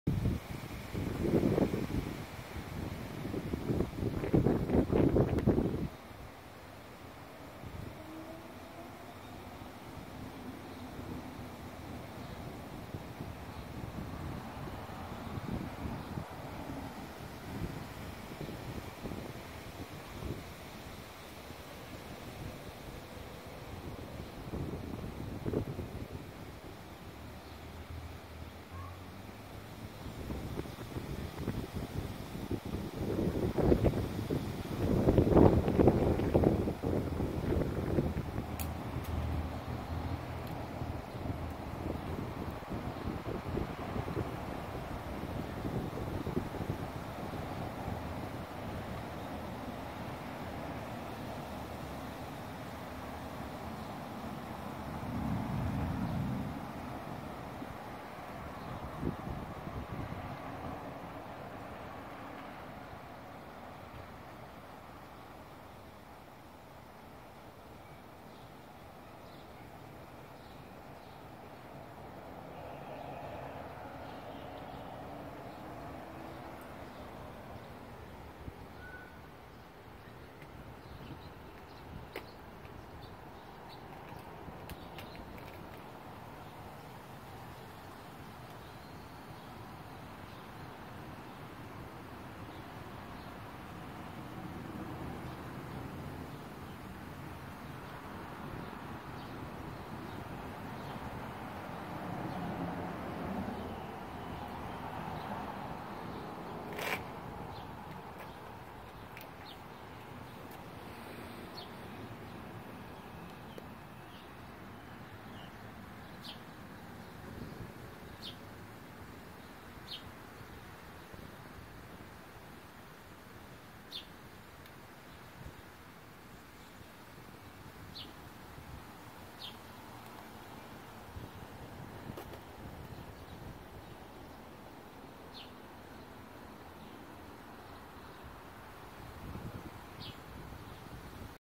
Eastern Black Swallowtail is in a hurry.
First female of season discovers garden parsley just in time.